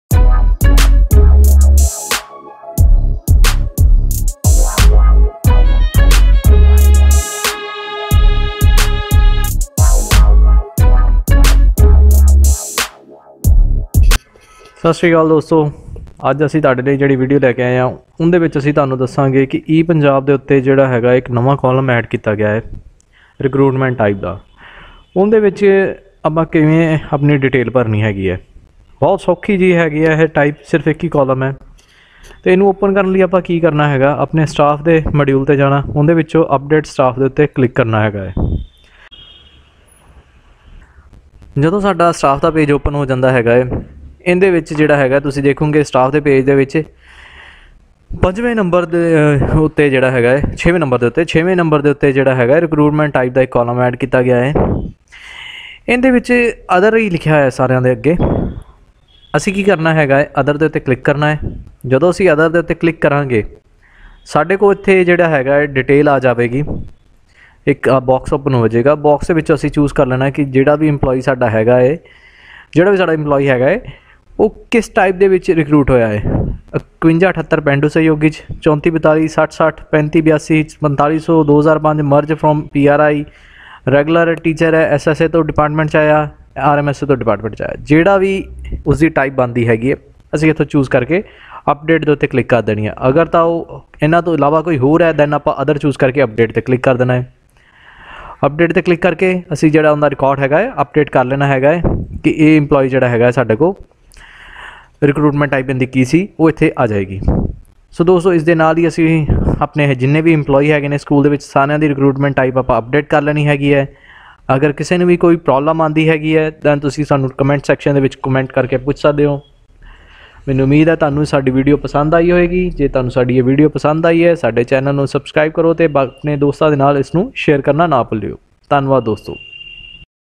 HOW TO ADD RECRUITMENT TYPE ON EPUNJAB WEBPORTAL (BY: TECHNICAL FOODIE)
IN THIS VIDEO WE HAVE EXPLAINED HOW TO ADD RECRUITMENT TYPE ON EPUNJAB WEBPORTAL.

1. OPEN STAFF UPDATE PAGE.
2. CLICK RECRUITMENT TYPE LINK AND UPDATE DATA.